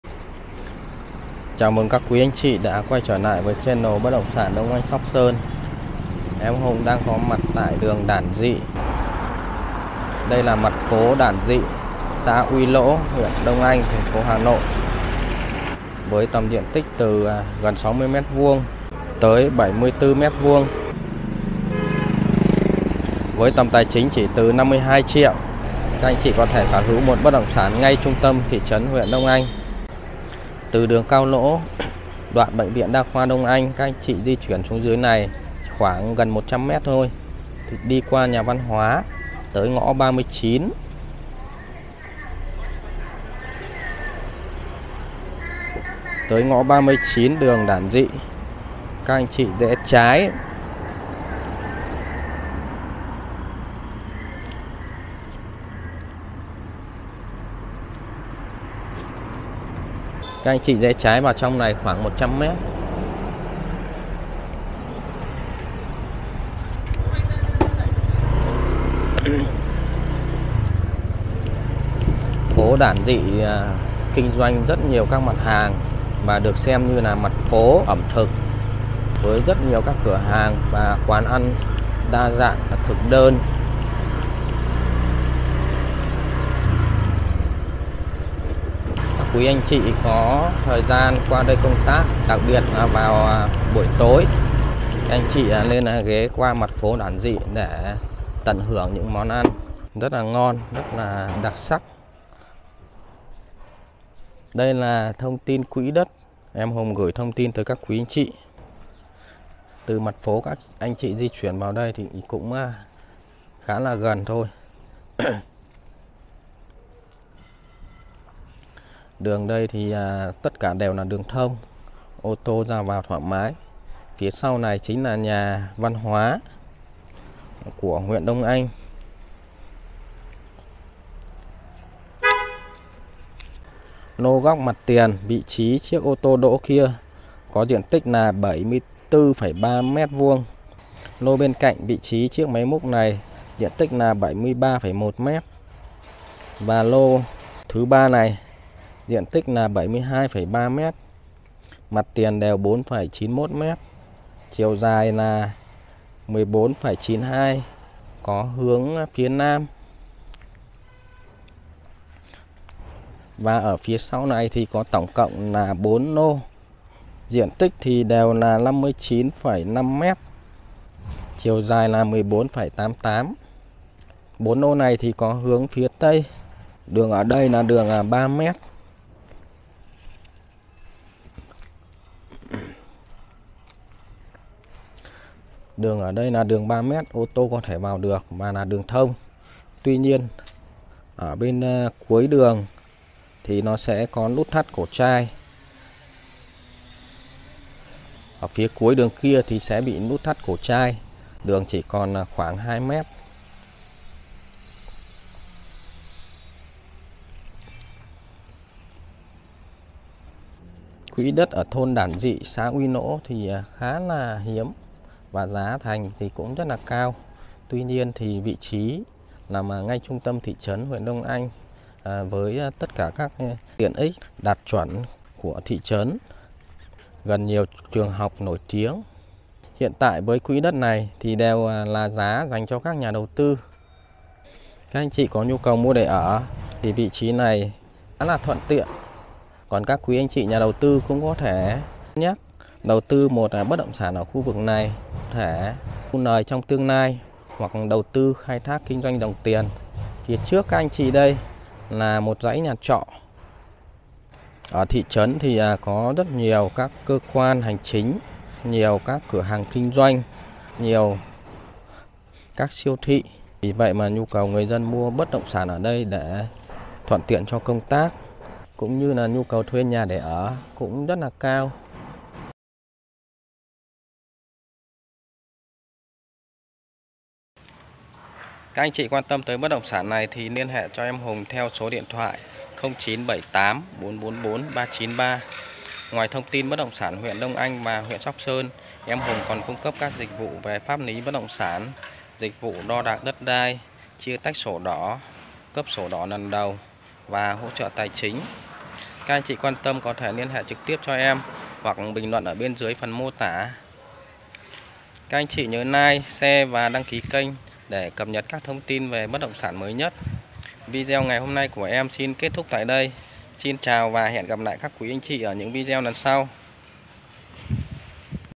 Đất Đản Dị, Đông Anh, Hà Nội, giá chỉ từ 52 triệu
Siêu phẩm mới phân lô tại Đản Dị, Uy Nỗ, Đông Anh, HN.

Diện tích từ 60 m2, tới 74 m2, giá chỉ từ 52 triệu

Giá đầu tư LH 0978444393

Đường rộng 5m, ngõ thông xe ê tô vào tận trong nhà
Vị trí : Gần UBND huyện Đông Anh, Nhà Văn hóa huyện, BVĐK Đông Anh, trường Đảng, chợ Tó...